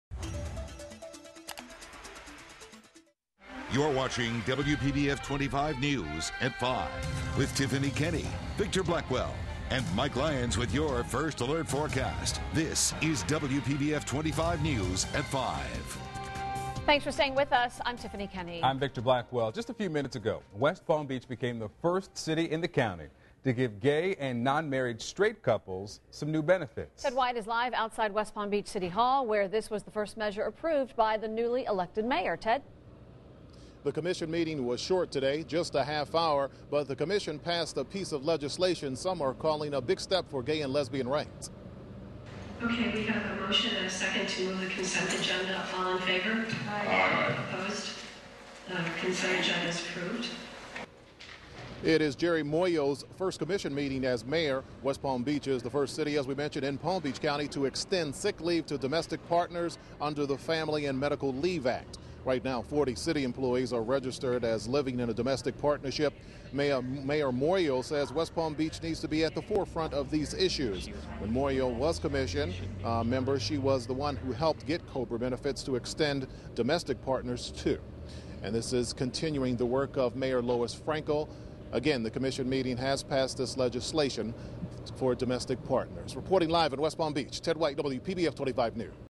Muoio's First Order Of Business: Benefits For Domestic Partners
Jeri Muoio's first order of business as the new mayor of West Palm Beach is to give new benefits to gay and straight domestic partners.